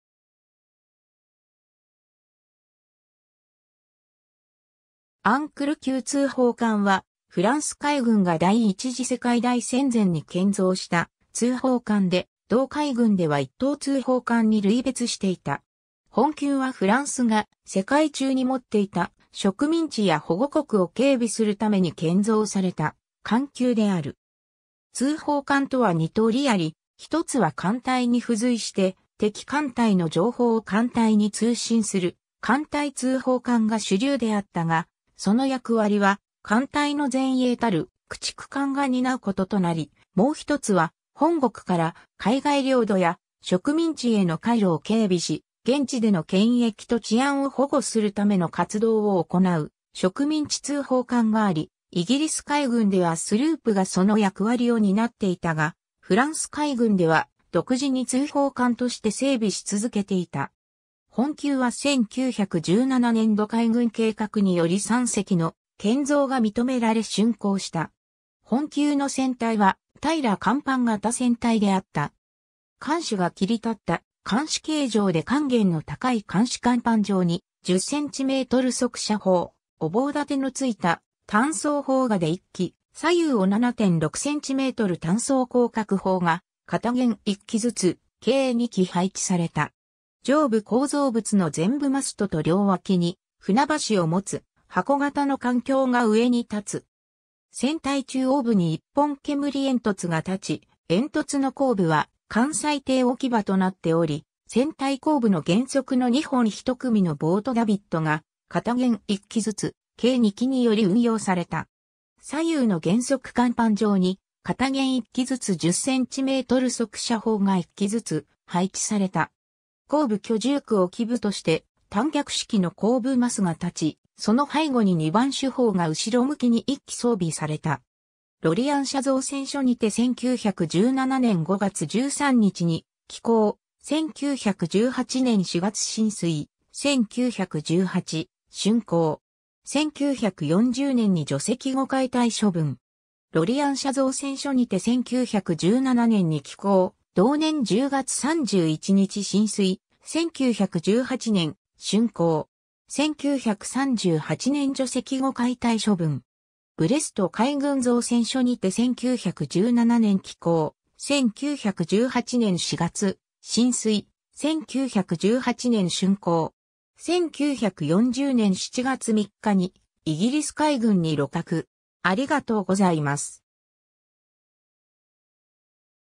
アンクル級通報艦
アンクル級通報艦（フランス語: Aviso colonial Classe Ancre）は、フランス海軍が第一次世界大戦前に建造した通報艦で同海軍では一等通報艦に類別していた。
本級はフランスが世界中に持っていた植民地（フランス植民地帝国）や保護国を警備するために建造された艦級である。
 通報艦とは二通りあり、一つは艦隊に付随して敵艦隊の情報を艦隊に通信する艦隊通報艦が主流であったが、その役割は艦隊の前衛たる駆逐艦が担う事となり、もう一つは本国から海外領土や植民地への海路を警備し、現地での権益と治安を保護するための活動を行う植民地通報艦があり、イギリス海軍ではスループがその役割を担っていたがフランス海軍では独自に通報艦（Aviso）として整備し続けていた。
 本級は1917年度海軍計画により3隻の建造が認められ竣工した。
 本級の船体は平甲板型船体であった。
艦首が切り立った艦首形状で乾舷の高い艦首甲板上に「10cm（45口径）速射砲」を防楯の付いた単装砲架で1基、左右を7.6cm単装高角砲が片舷1基ずつ計2基配置された。
上部構造物の前部マストと両脇に船橋をもつ箱型の艦橋が上に立つ。
 船体中央部に1本煙煙突が立ち、煙突の後部は艦載艇置き場となっており、船体後部の舷側の2本1組のボート・ダビッドが片舷1基ずつ計2基により運用された。
左右の舷側甲板上に片舷1基ずつ10cm速射砲が1基ずつ配置された。
後部居住区を基部として単脚式の後部マスが立ち、その背後に2番主砲が後ろ向きに1基装備された。
 ロリアン社造船所にて1917年5月13日に起工、1918年4月進水、1918竣工。
1940年に除籍後解体処分。
 ロリアン社造船所にて1917年に起工、同年10月31日進水、1918年竣工。
1938年除籍後解体処分。
 ブレスト海軍造船所にて1917年起工、1918年4月進水、1918年竣工。
1940年7月3日にイギリス海軍に鹵獲。